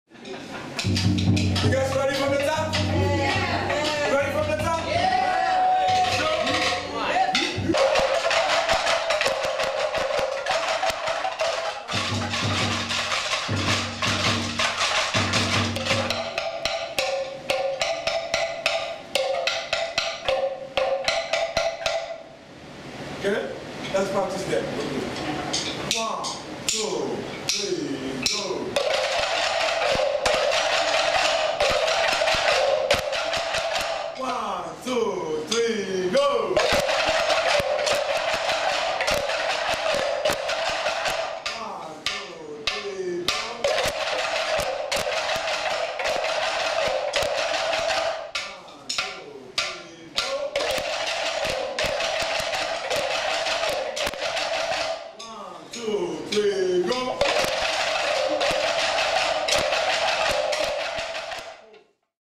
Ibrahima "Kolipe" Camara teaches Krin at Wula Drum
Ibrahima "Kolipe" Camara teaches Krin at Wula Drum's Percussion Center of New York!

Don't Miss Kolipe's Krin workshop this Saturday, August 6th at The  Percussion Center of New York located at 47-09 30th St Long Island City, Queens, 11101. Kolipe will also be teaching a Djembe and Dundun workshop on Saturday.

Djembe and Dundun 12:00 - 1:30pm
Kyrin 1:30 - 3:00pm

*Both workshops are $40 / Single workshops is $25*